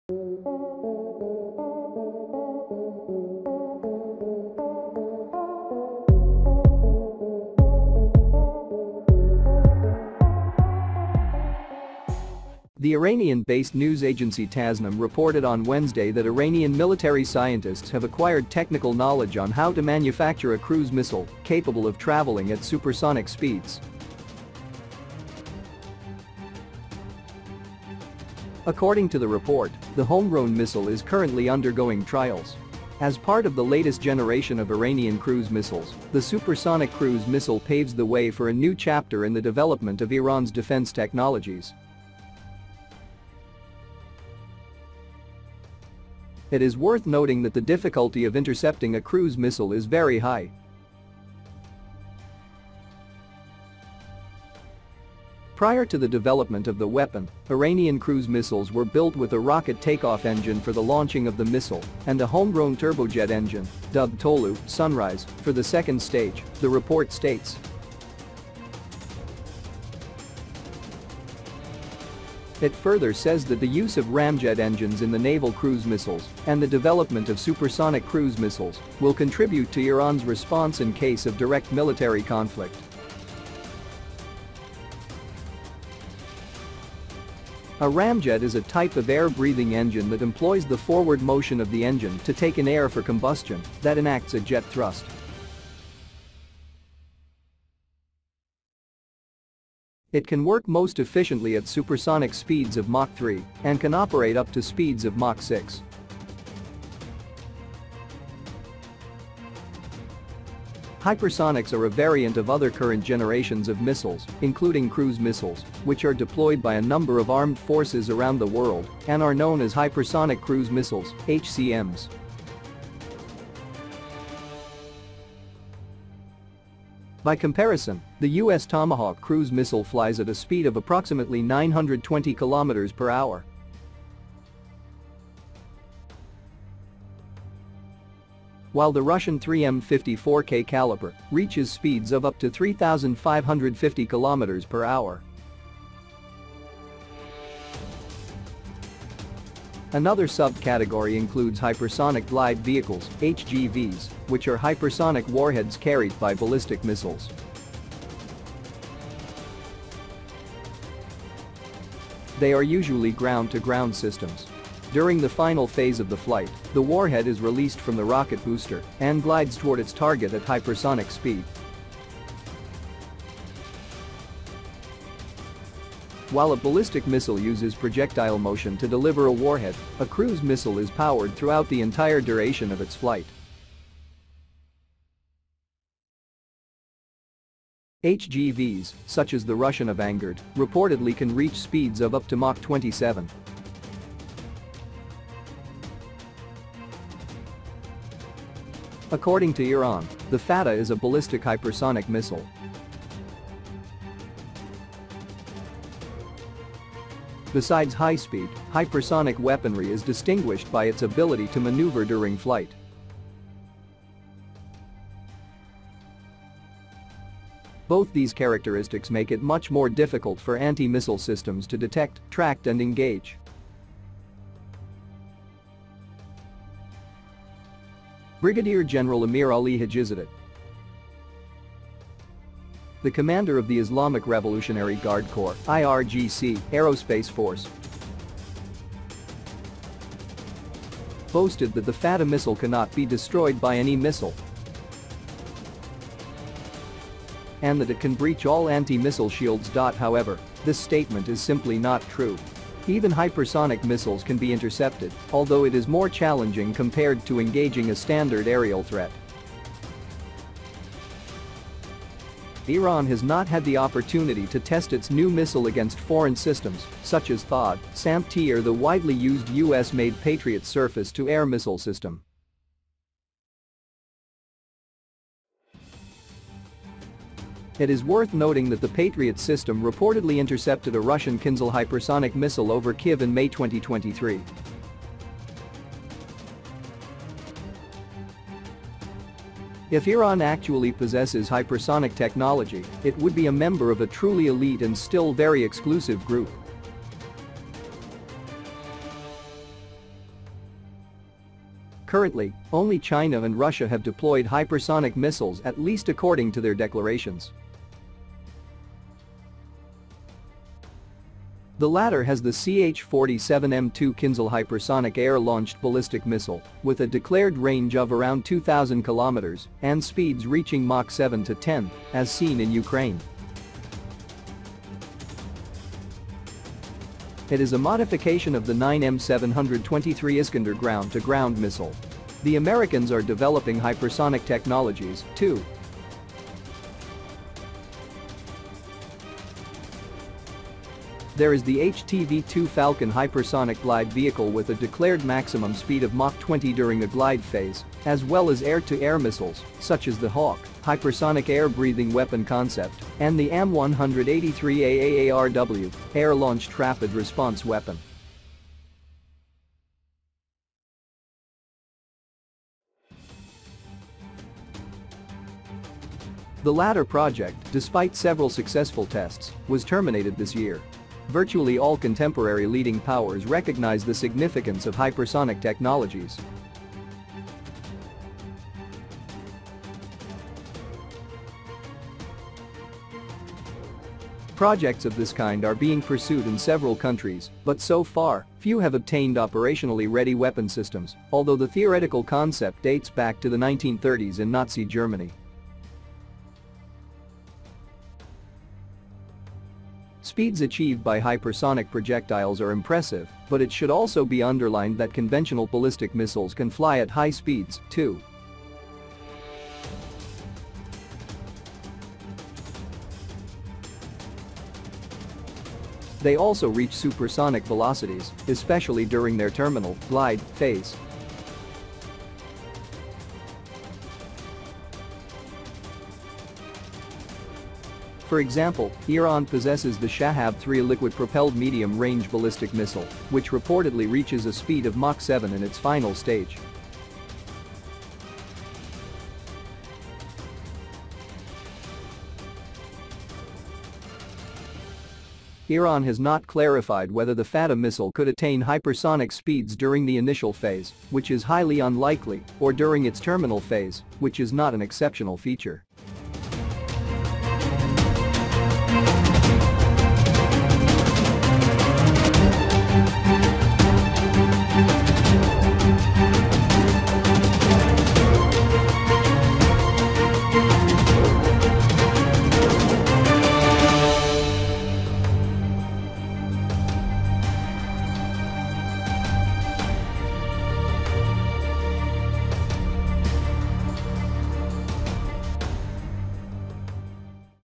Iran makes breakthrough in manufacture of supersonic cruise missile
Iranian military scientists have acquired technical knowledge on how to manufacture a cruise missile capable of traveling at supersonic speeds.